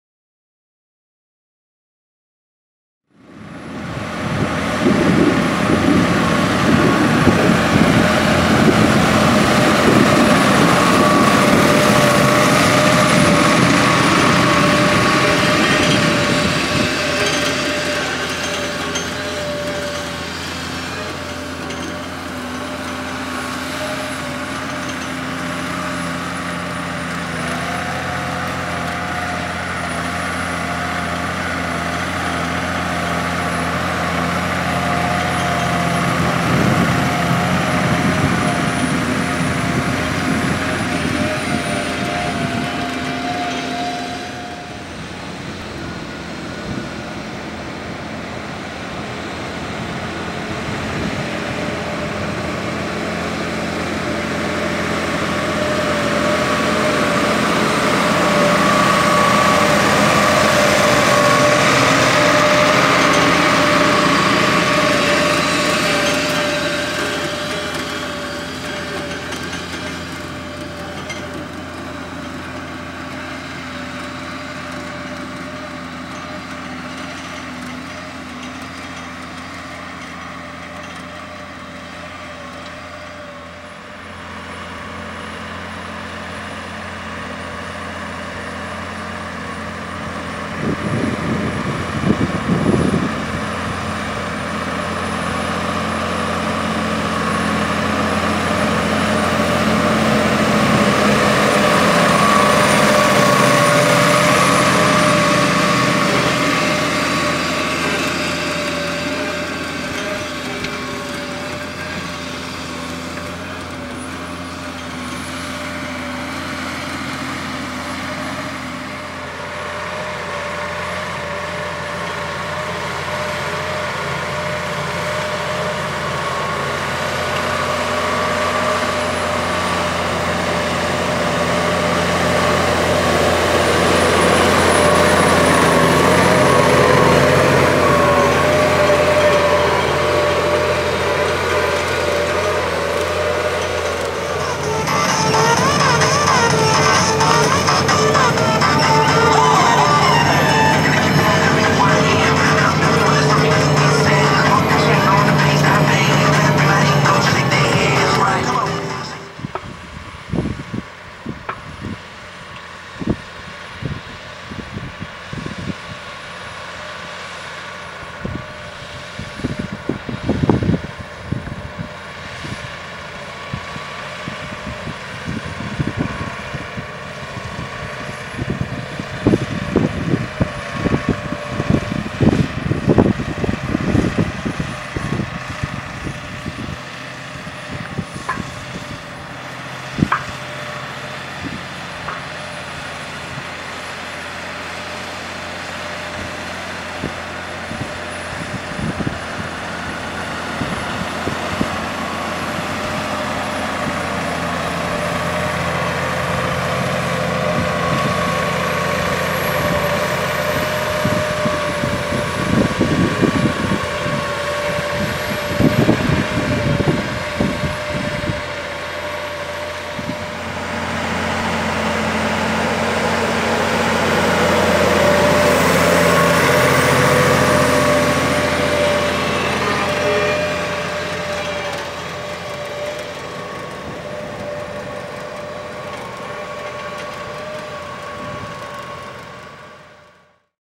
Livellamento black Puma215 + Fontana
Livellamento di una risaia il 27/03/14 a Pieve Emanuele.
Macchine nel video:
-Case Ih Puma 215 Black
-Livella Fontana